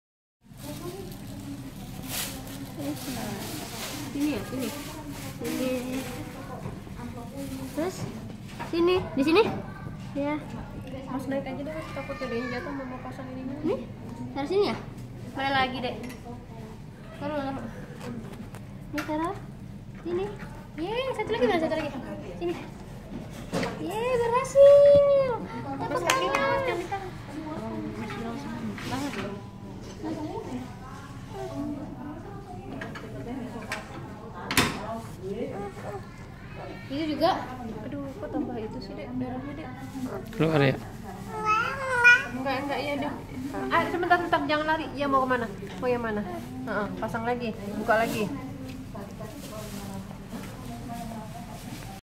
Puzzle Hello Kitty
Bermain puzzle hello kitty saat Syakira dakit typus seminggu di rumah sakit Permata Depok.